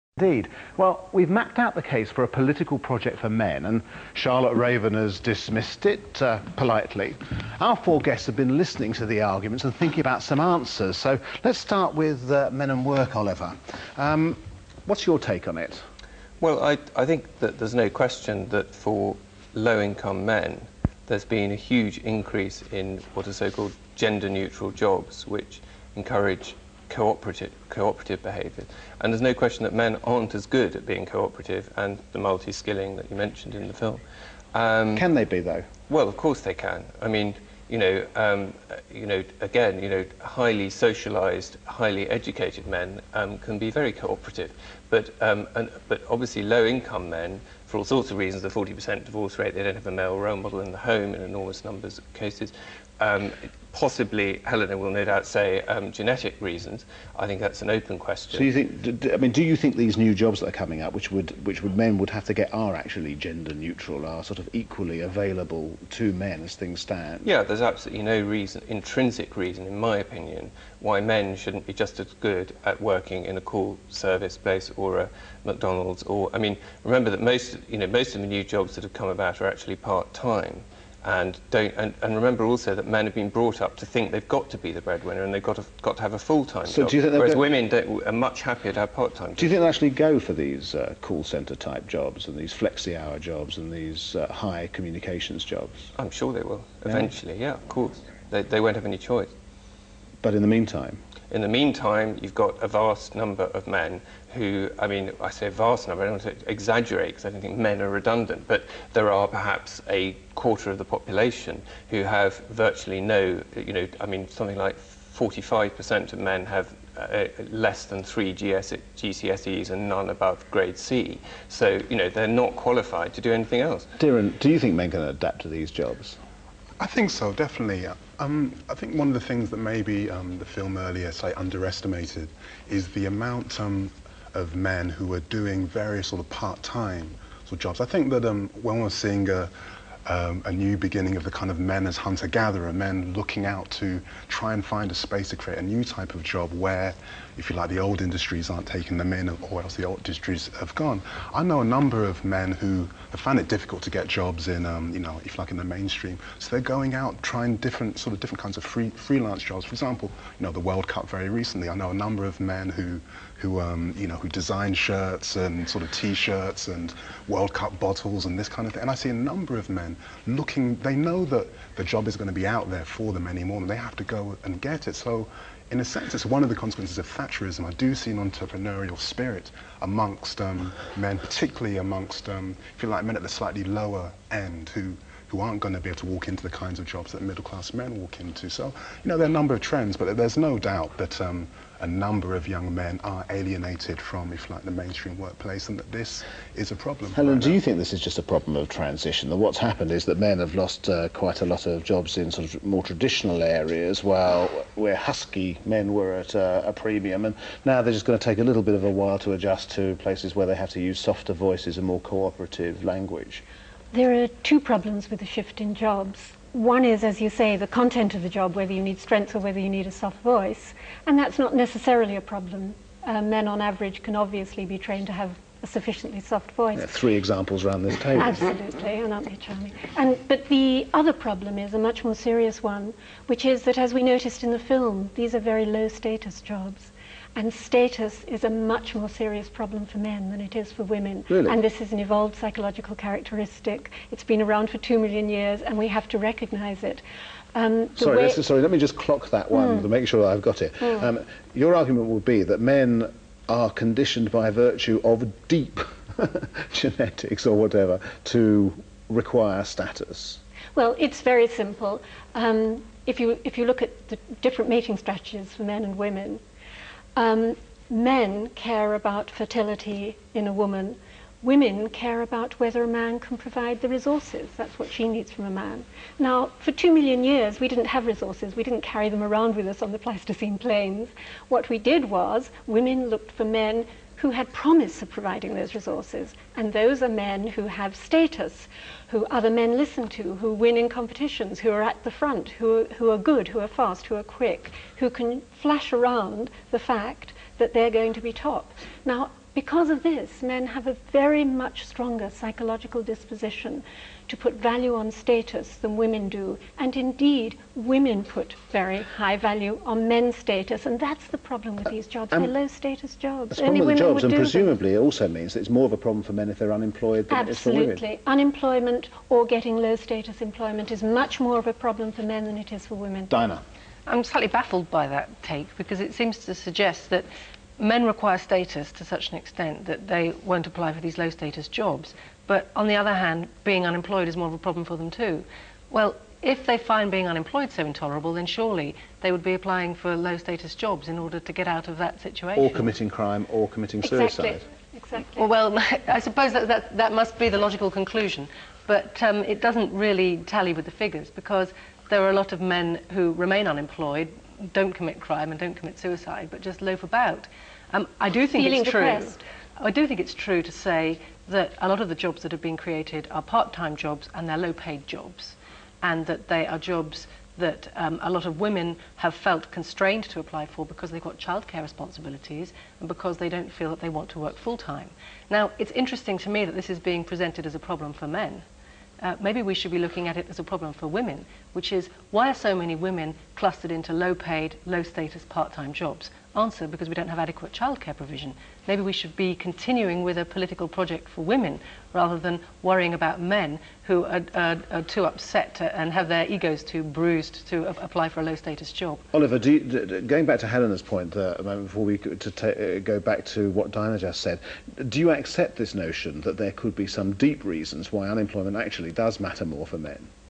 #4 Crisis of Masculinity... Do Men need Special Help? - #4 of 6
With boys falling behind girls at school, men finding their traditionally-male employment avenues dwindling, & fathers becoming increasingly excluded from the family, do men require specially-tailored help to keep up with women? - part 4 of 6